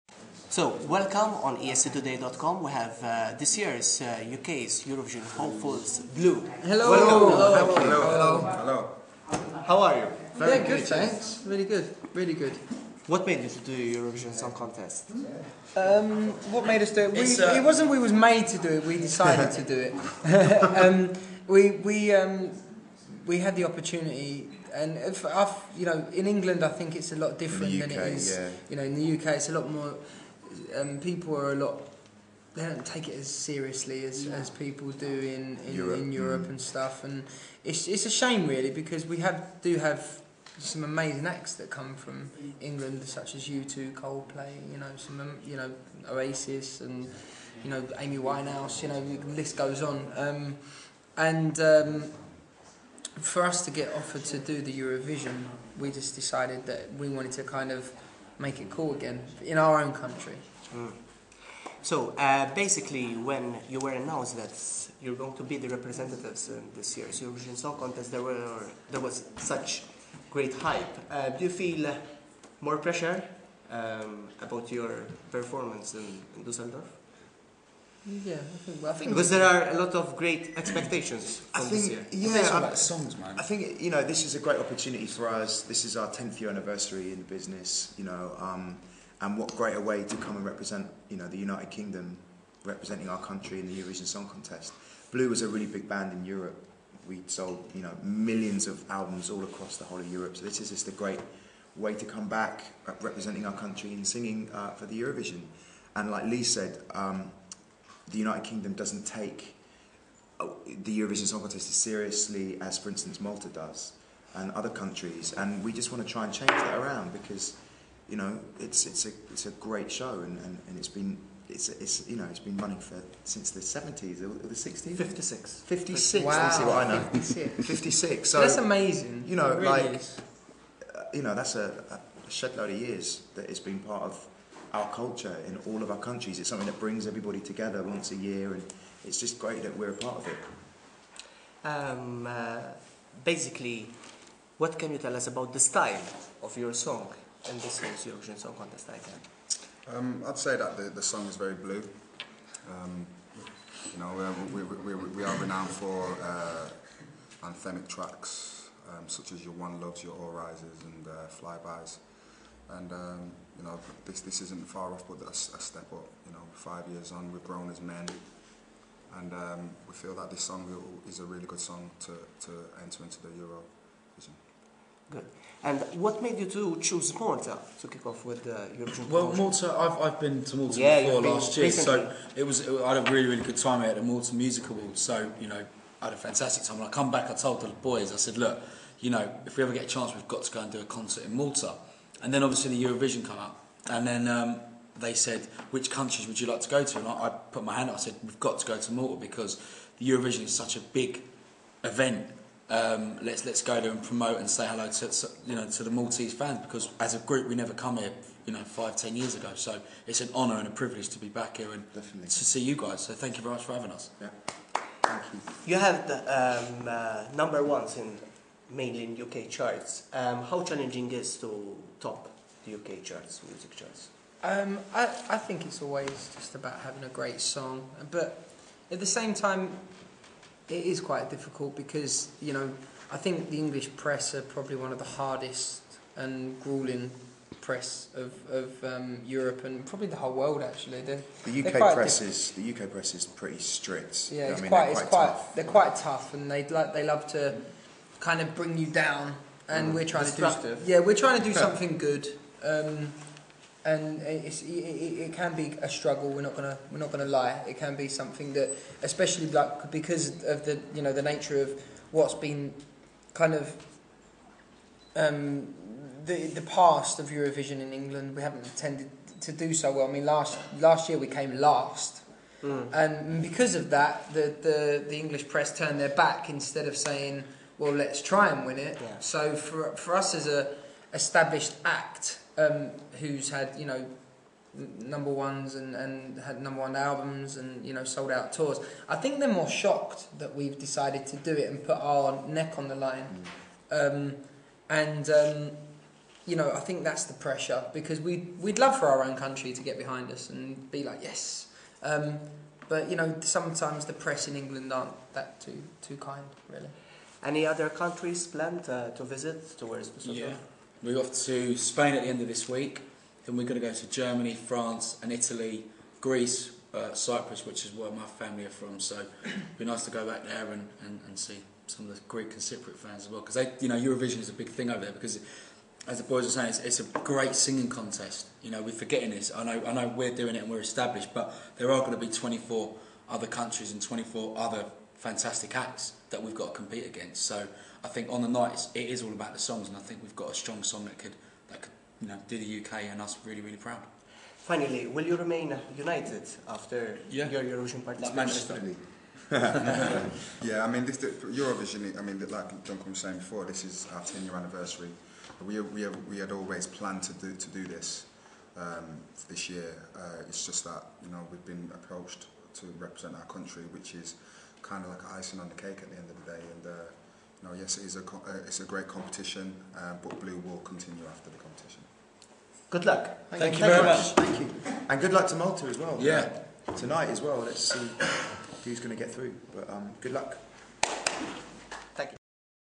Interview: Blue speaks to esctoday.com
Interview: UK's 2011 Eurovision representatives Blue speaks to esctoday.com